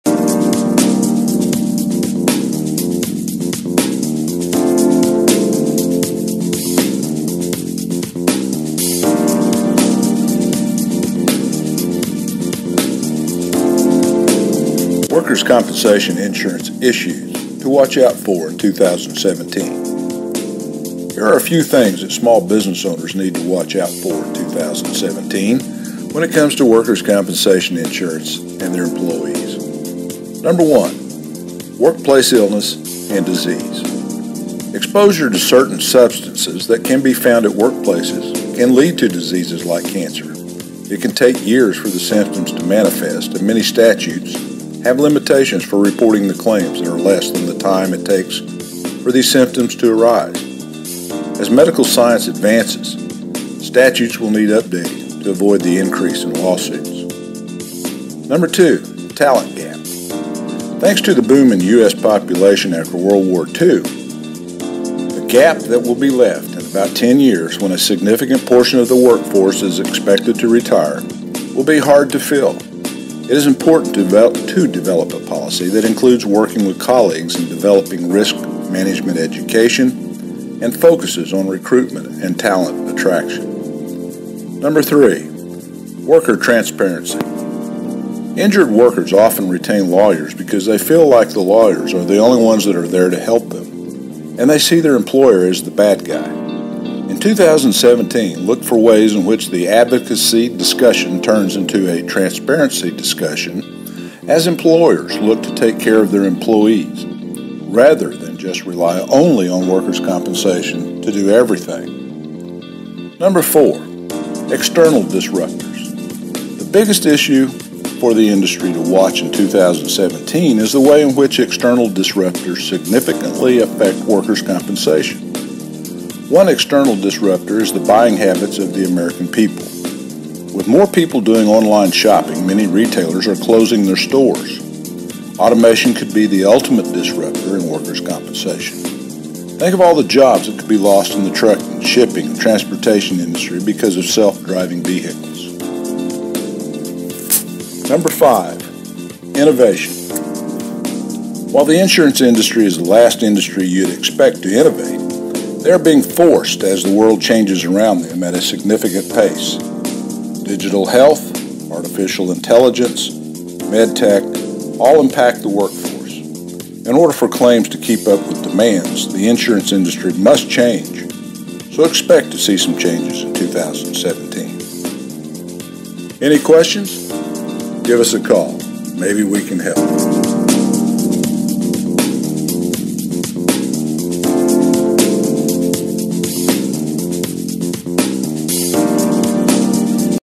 "Workers Compensation Issues to Watch Out for in 2017"
There are a few things that small business owners need to watch out for in 2017 when it comes to workers’ compensation insurance and their employees.  Every company in Oklahoma that has an employee must carry workers compensation.  Competitive pricing is available through the Oak Tree Agency in Edmond, Oklahoma.